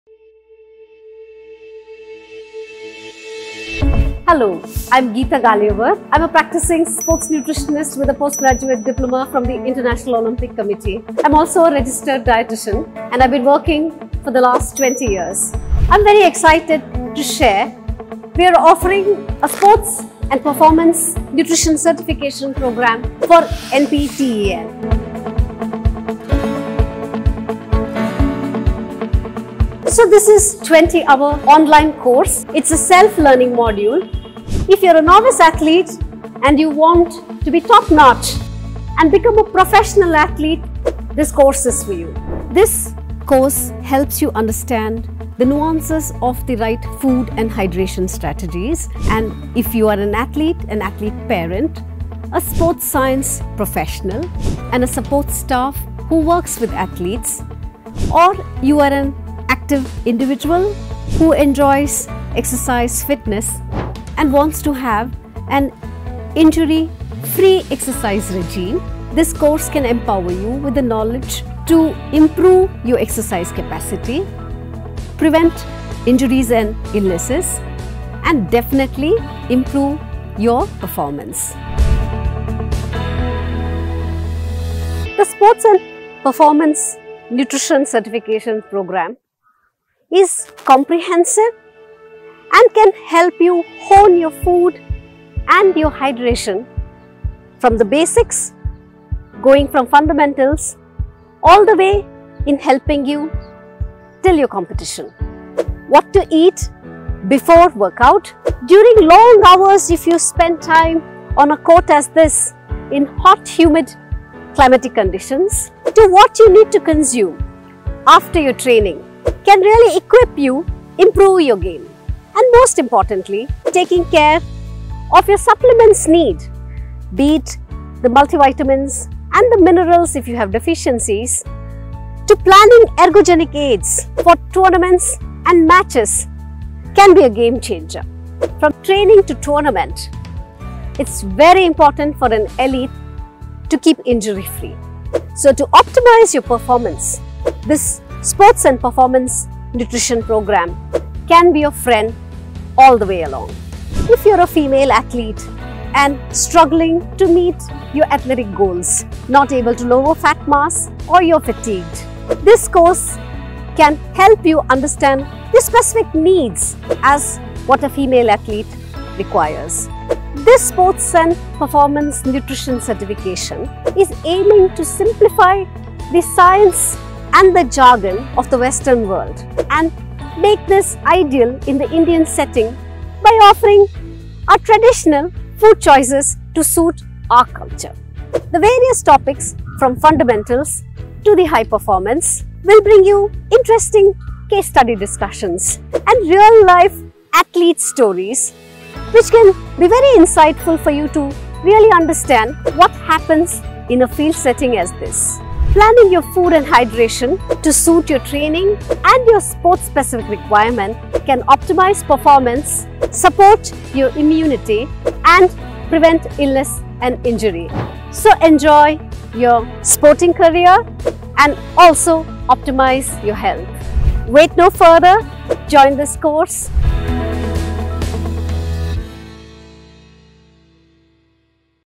Course Introduction | Sports and Performance Nutrition | Geetha Ghaliyavar | NPTEL | IIT Madras
Sports and Performance Nutrition

Sports nutritionist
Geetha Ghaliyavar
(PG Diploma -International Olympic Committee, UK )
Registered Dietitian
Certified Diabetes Educator
IBS certification ( Monash University, Australia)

ABOUT THE COURSE :
Sports Nutrition is a branch of Nutritional Science that deals with the interaction of human nutrition with exercise performance. Through this course, we aim to connect physical activity, nutrition and health by translating the nutritional goals of an athlete into dietary strategies. By completing the course, you’d have a basic understanding of how nutrients can be manipulated to boost performance and how to incorporate traditional and Indian cuisine into sports nutrition.